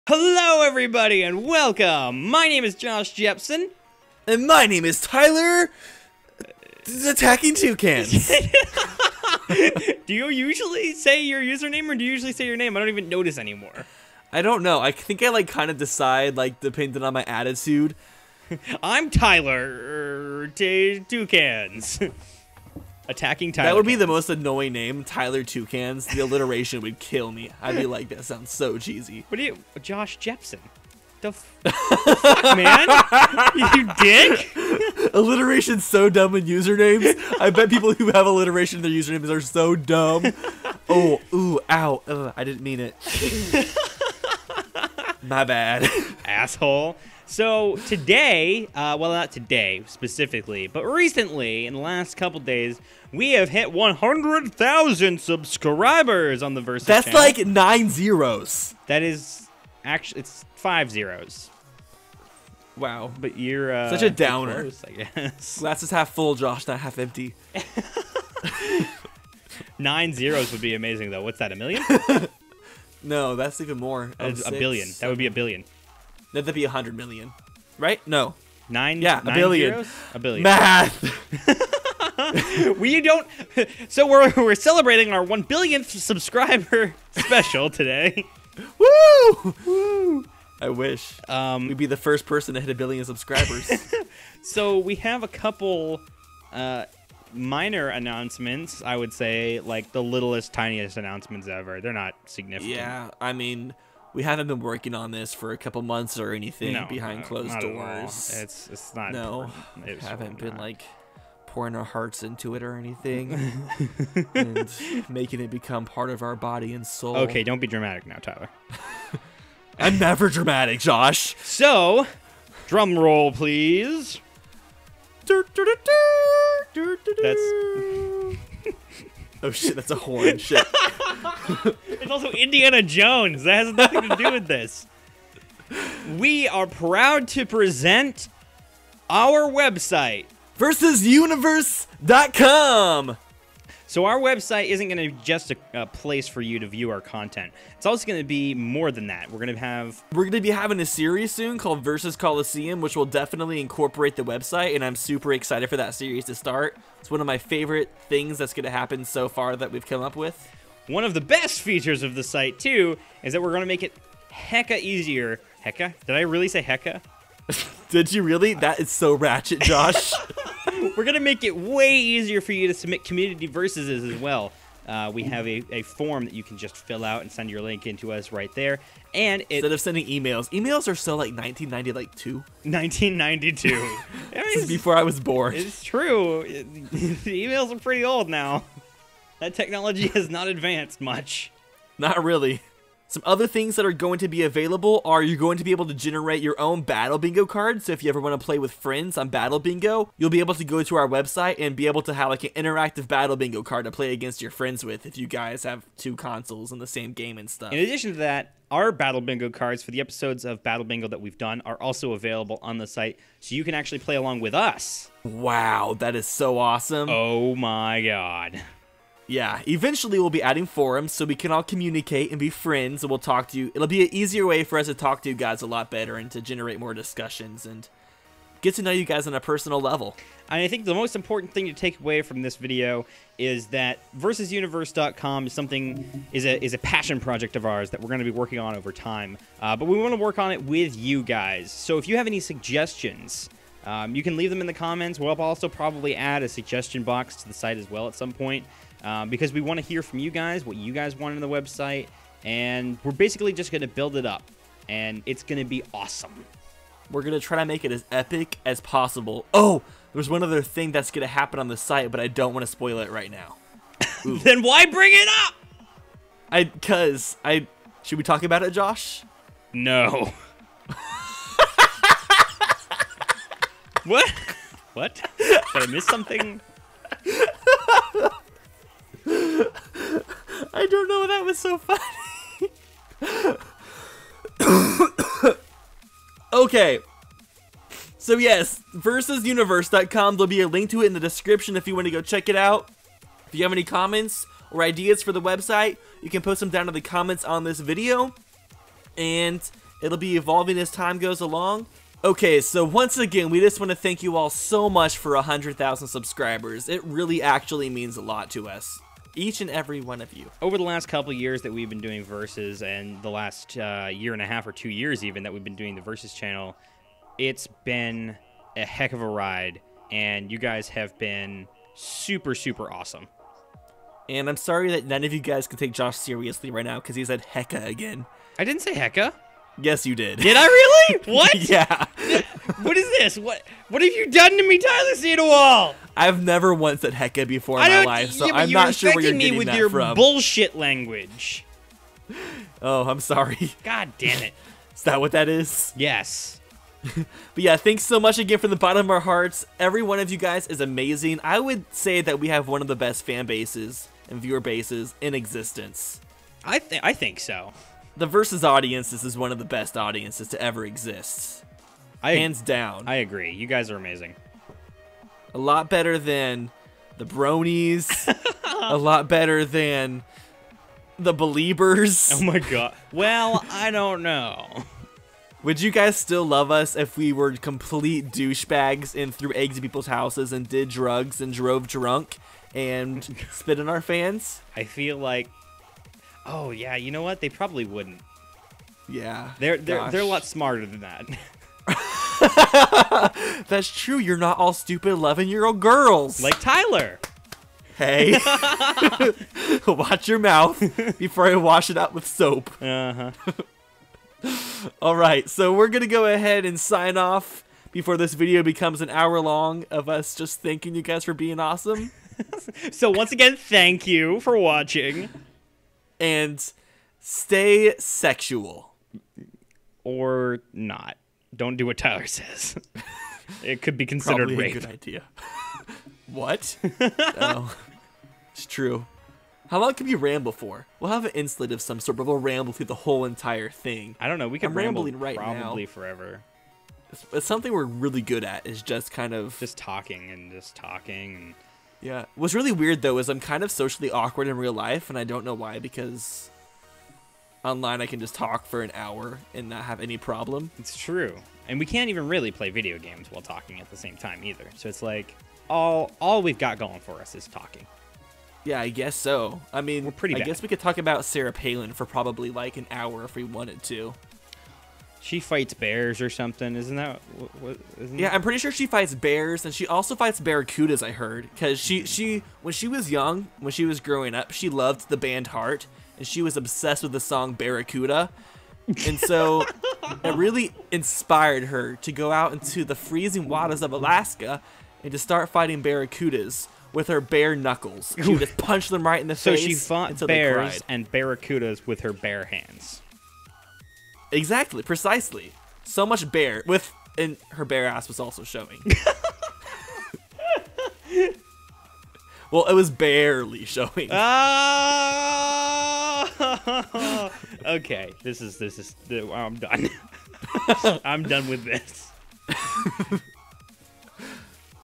100,000 Subscriber Special - Thank You! - New Website!
Thanks to every single one of you for being so supportive of everything we do! We love all of you and hope that this year ends up being great!

Now check out the website dawggg!